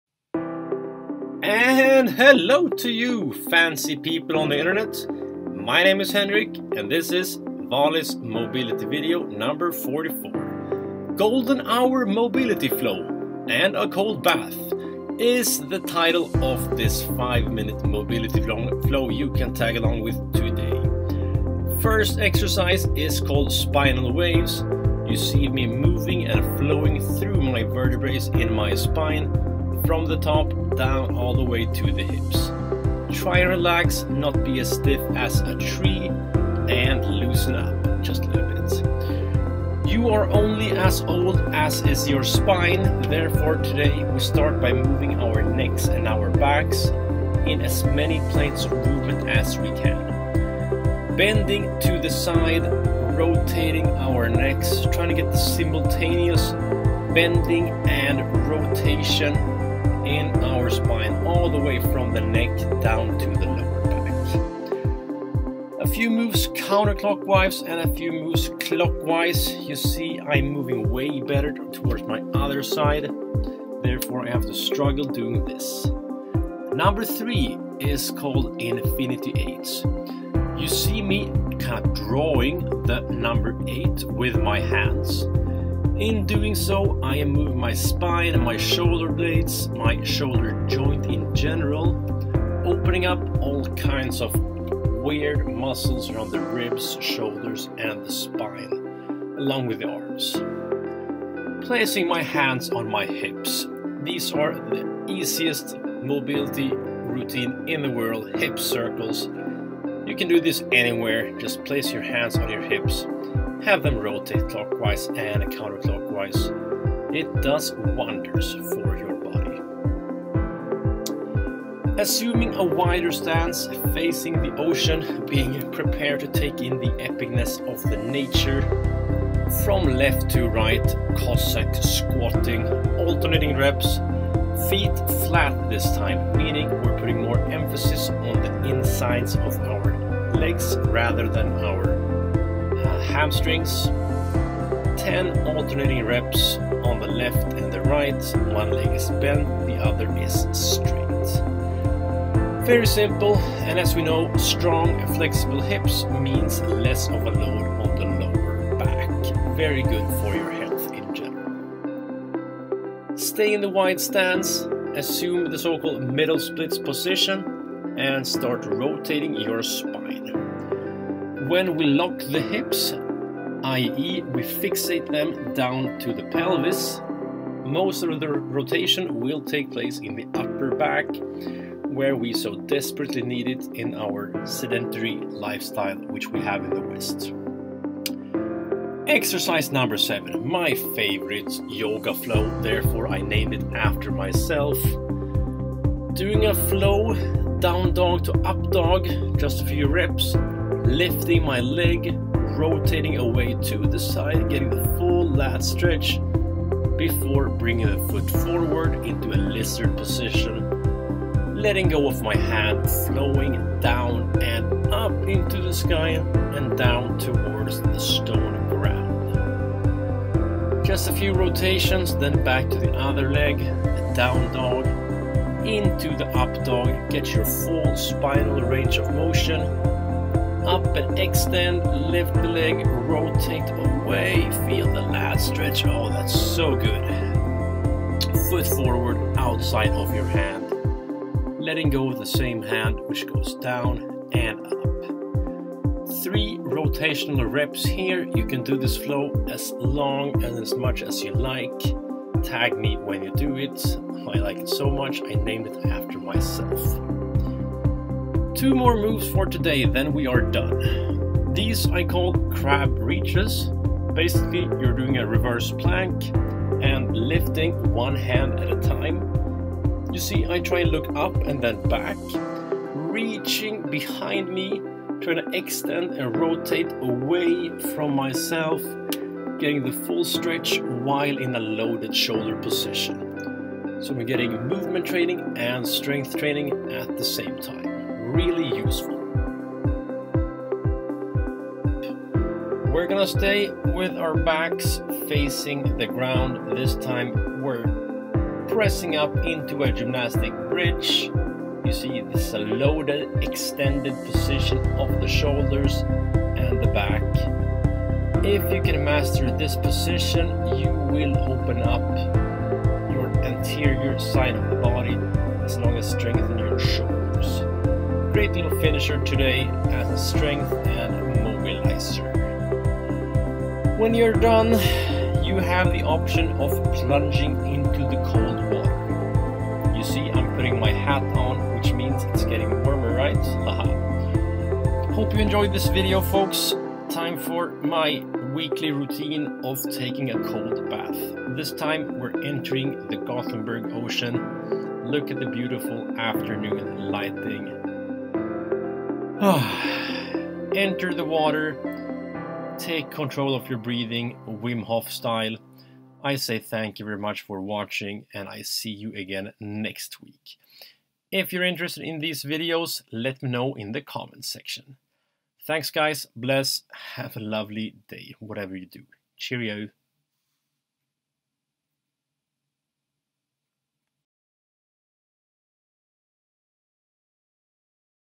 5 MIN MOBILITY AND A COLD BATH - ValisMobilityVideo (2020-w44)
Hey!

Good habits require courage, discipline and consistency! Today, I manage to engage in two things that are beneficial to the human spirit and body, namely a mobility flow and a cold bath! Yes, it is more comfortable to be wrapped up into a blanket and sipping on a coffee all day, but most epicness seldom occur within the comfort zone.

In the video, I'm doing a full body mobility routine possibly with a bit more emphasis on the spine. Afterwards, I descend into the ocean and take 10 deep breaths. Try it out and I guarantee you it will wake you up more than any cup of coffee :-)